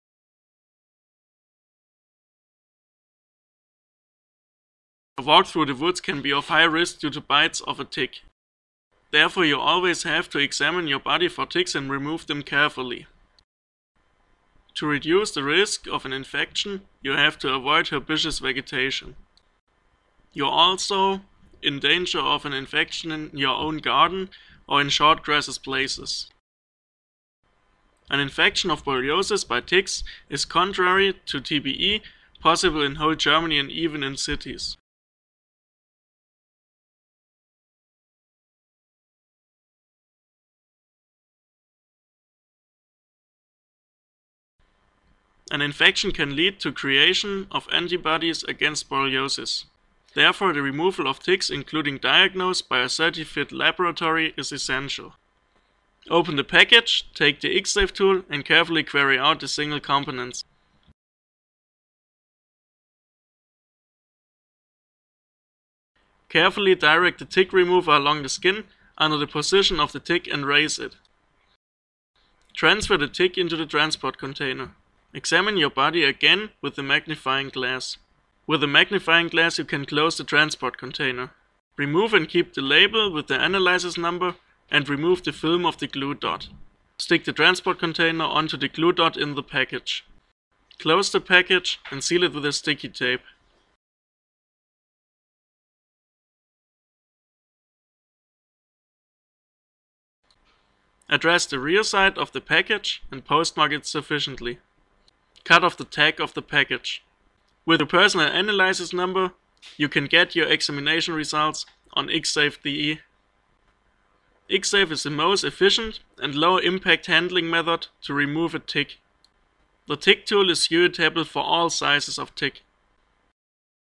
IxSave - Zeckenentfernung & Labordiagnose
What the IxSave is for and how it's used.